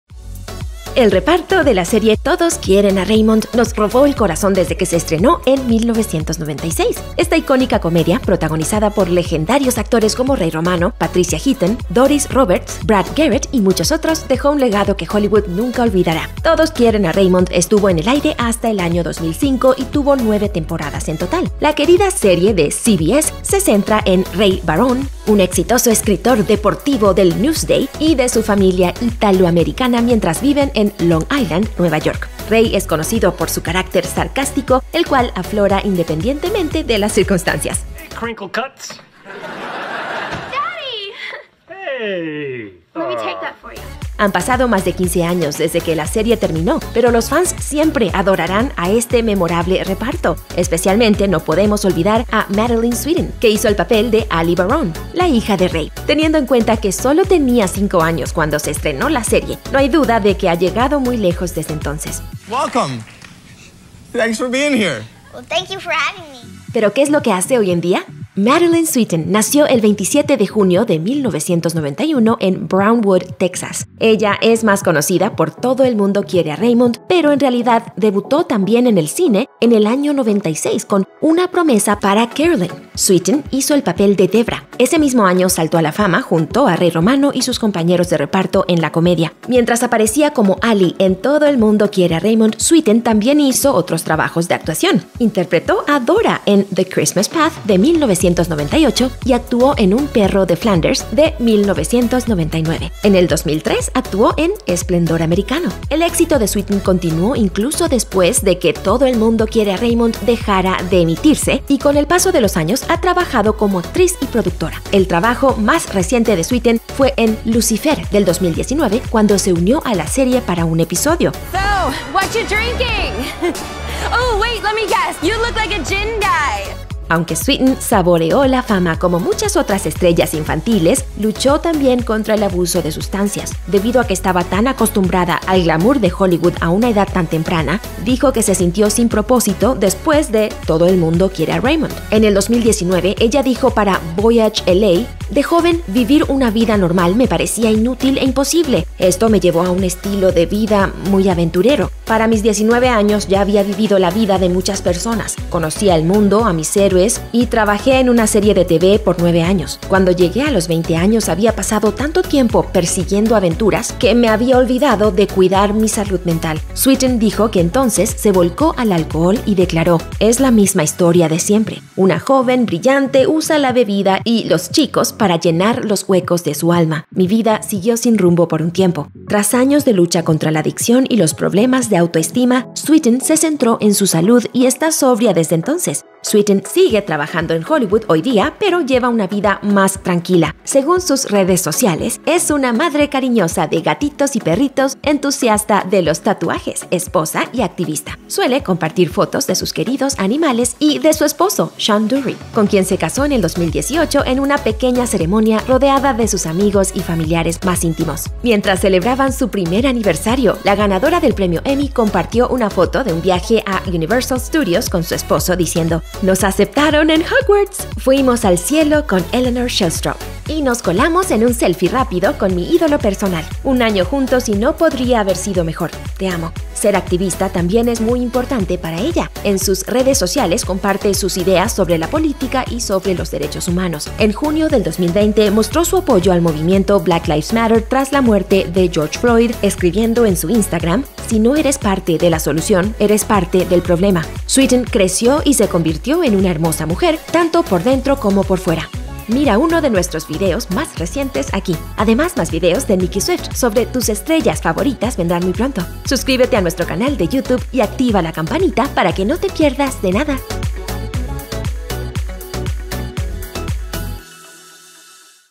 La Hija Televisiva De Ray Romano Luce Hermosa Ahora A Sus 20 Años
El reparto de la serie Todos Quieren A Raymond nos robó el corazón desde que se estrenó en 1996. Esta icónica comedia, protagonizada por legendarios actores como Ray Romano, Patricia Heaton, Doris Roberts, Brad Garrett y muchos otros, dejó un legado que Hollywood nunca olvidará.

Todos Quieren A Raymond, estuvo en el aire hasta el año 2005 y tuvo 9 temporadas en total. La querida serie de la CBS se centra en Ray Barone, un exitoso escritor deportivo del Newsday, y su familia italoamericana mientras viven en Long Island, Nueva York. Ray era conocido por su carácter sarcástico, el cual aflora independientemente de las circunstancias.

Han pasado más de 15 años desde que la serie terminó, pero los fans siempre adorarán a este memorable reparto. Especialmente no podemos olvidar a Madylin Sweeten, que hizo el papel de Ally Barone, la hija de Ray. Teniendo en cuenta que sólo tenía 5 años cuando se estrenó la serie, no hay duda de que ha llegado muy lejos desde entonces.

Pero, ¿qué es lo que hace hoy en día?

Madylin Sweeten nació el 27 de junio de 1991 en Brownwood, Texas. Ella es más conocida por Todo el Mundo Quiere A Raymond, pero en realidad debutó también en el cine en el año 96 con Una promesa para Carolyn. Sweeten hizo el papel de Debra. Ese mismo año, saltó a la fama junto a Ray Romano y sus compañeros de reparto en la comedia.

Mientras aparecía como Ally en Todo El Mundo Quiere A Raymond, Sweeten también hizo otros trabajos de actuación. Interpretó a Dora en The Christmas Path, de 1998, y actuó en Un perro de Flanders, de 1999. En el 2003 actuó en Esplendor Americano.

El éxito de Sweeten continuó incluso después de que Todo El Mundo Quiere A Raymond dejara de emitirse, y con el paso de los años ha trabajado como actriz y productora. El trabajo más reciente de Sweeten fue en Lucifer del 2019, cuando se unió a la serie para un episodio.

Aunque Sweeten saboreó la fama, como muchas otras estrellas infantiles, luchó también contra el abuso de sustancias. Debido a que estaba tan acostumbrada al glamour de Hollywood a una edad tan temprana, dijo que se sintió sin propósito después de Todo el Mundo Quiere A Raymond.

En el 2019, ella dijo para Voyage LA,

"De joven, vivir una vida normal me parecía inútil e imposible. [...] Esto me llevó a un estilo de vida muy aventurero. Para mis 19 años ya  había vivido la vida de muchas personas. Conocí al mundo, a mis héroes y trabajé en una serie de TV por nueve años. Cuando llegué a los veinte años, había pasado tanto tiempo persiguiendo aventuras que me había olvidado de cuidar mi salud mental."

Sweeten dijo que entonces se volcó al alcohol, y declaró,

"Es la misma historia de siempre, una joven brillante usa la bebida y los chicos para llenar los huecos de su alma. [...] Mi vida siguió sin rumbo por un tiempo."

Tras años de lucha contra la adicción y los problemas de autoestima, Sweeten se centró en su salud y está sobria desde entonces.

Sweeten sigue trabajando en Hollywood hoy día, pero lleva una vida más tranquila. Suele compartir fotos de sus queridos animales y de su esposo, Sean Durrie, con quien se casó en el 2018 en una pequeña ceremonia rodeada de amigos y familiares más íntimos.

Mientras celebraban su primer aniversario, la ganadora del premio Emmy compartió una foto  de un viaje a Universal Studios con su esposo, diciendo,

"¡Nos aceptaron en Hogwarts, fuimos al 'cielo' con Eleanor Shelstrop y nos colamos en un selfie rápido con mi ídolo personal! Un año juntos y no podría haber sido mejor, te amo."

En junio del 2020, mostró su apoyo al movimiento Black Lives Matter tras la muerte de George Floyd, escribiendo en su Instagram,

"¡Si no eres parte de la solución, eres parte del problema!"

Sweeten creció y se convirtió en una hermosa mujer, tanto por dentro como por fuera.